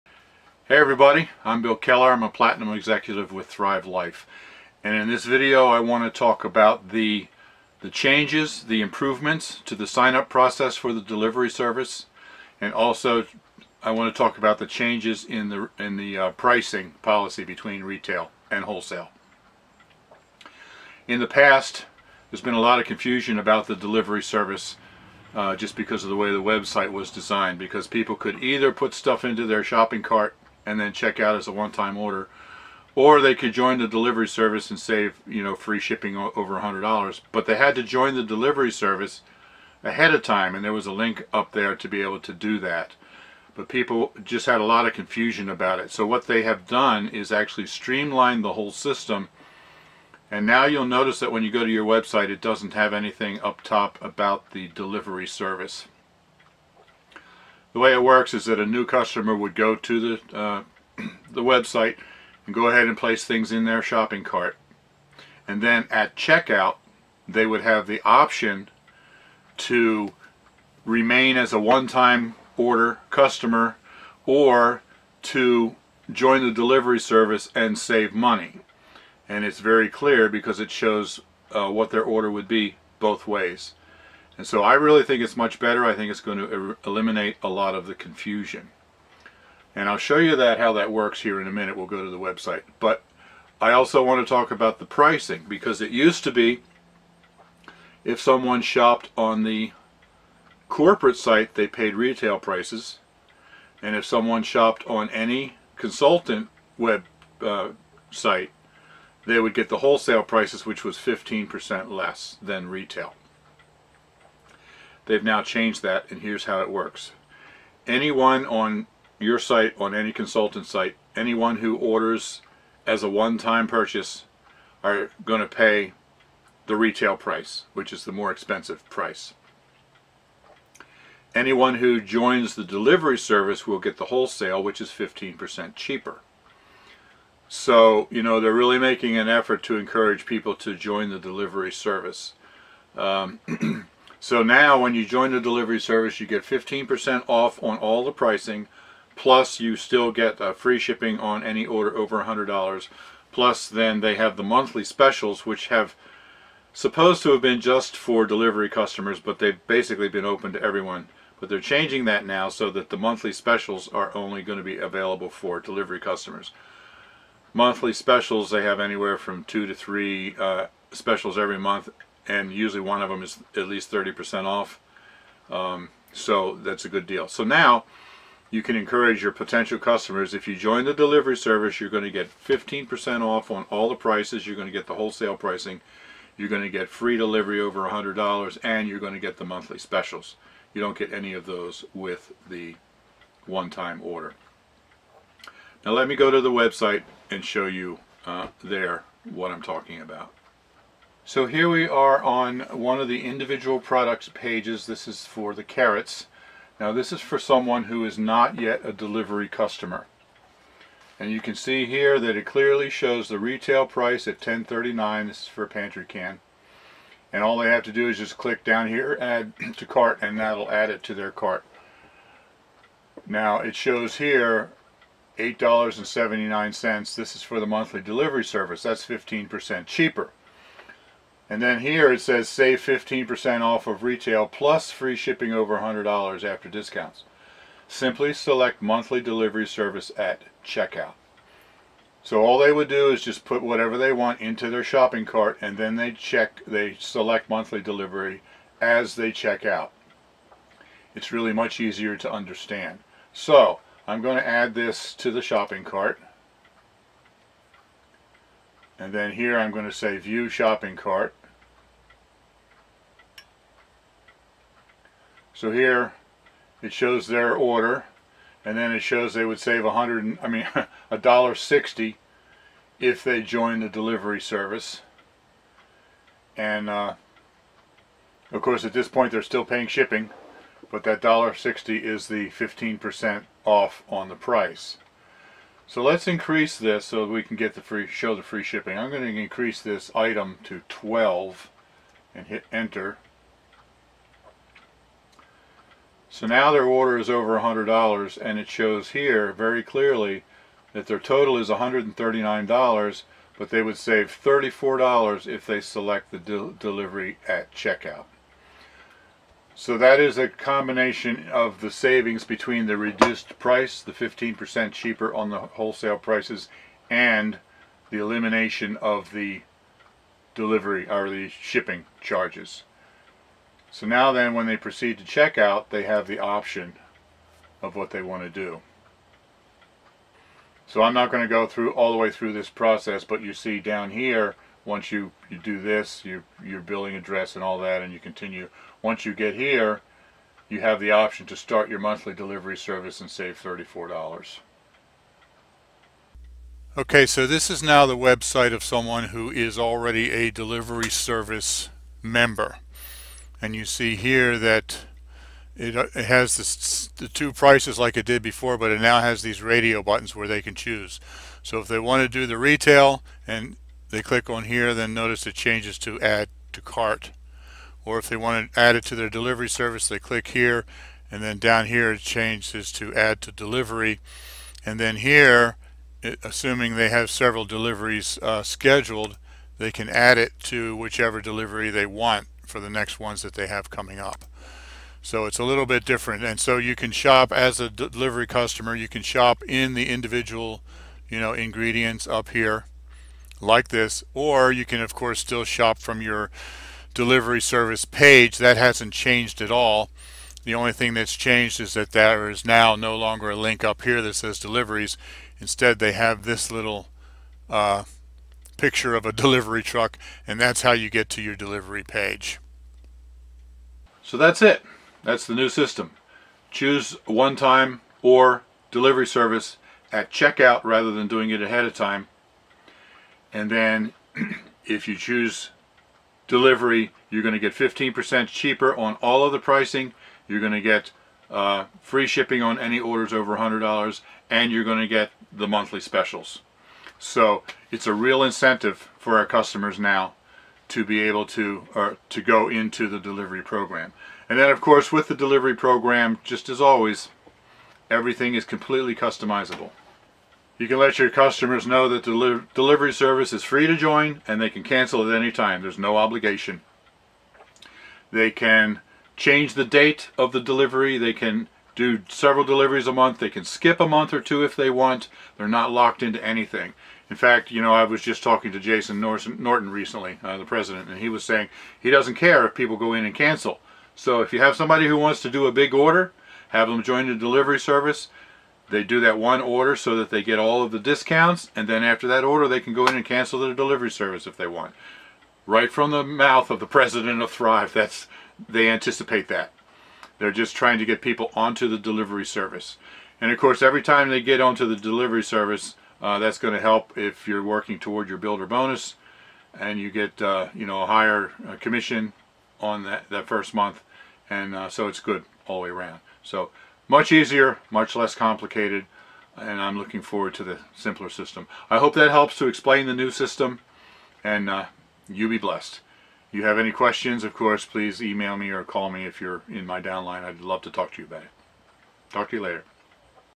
New Delivery and Wholesale/Retail Pricing
Explaining the new Delivery Service sign up process and the new policy on wholesale/retail pricing. (as of Sept 2019)